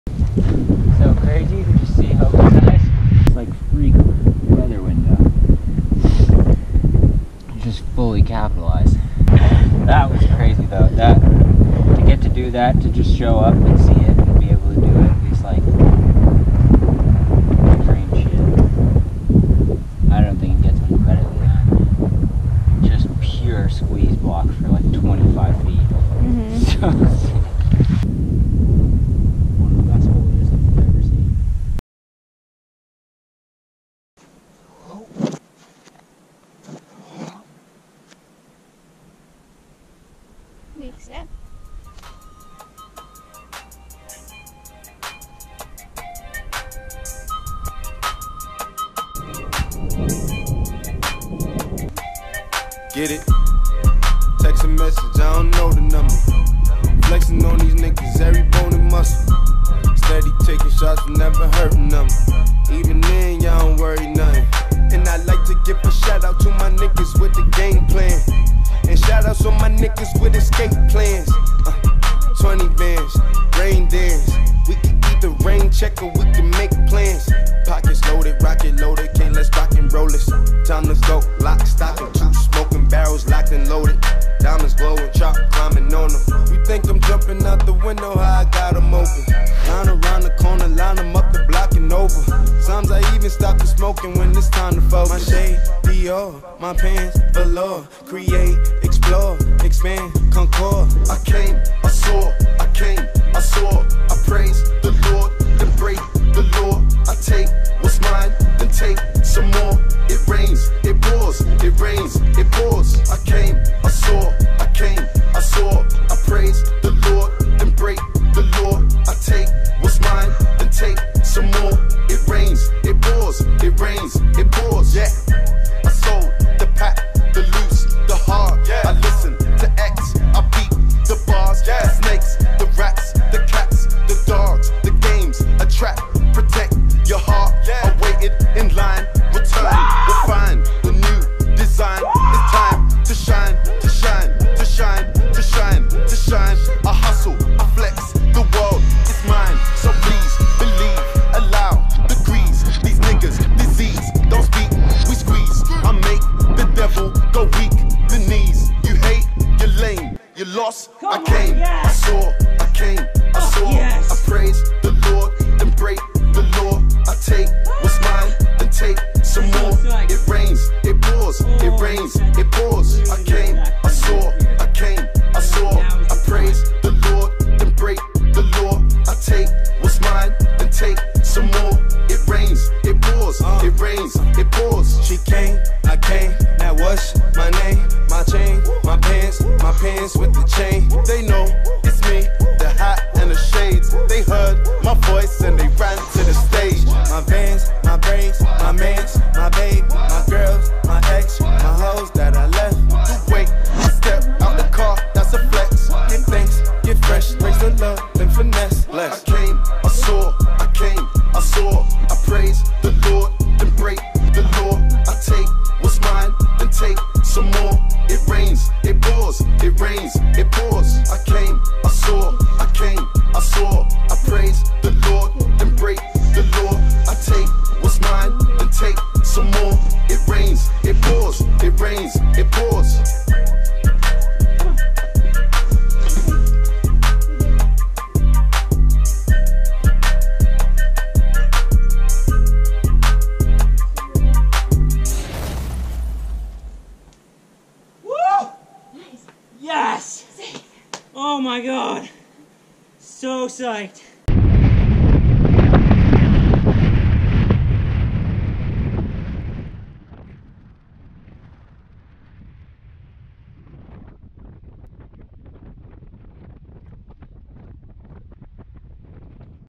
Roy in Reverse
A couple of amazing days in late September 2018 in reverse order... the FA of Rain Dance, a repeat of Calm Ethereal, and the FA of Mavericks.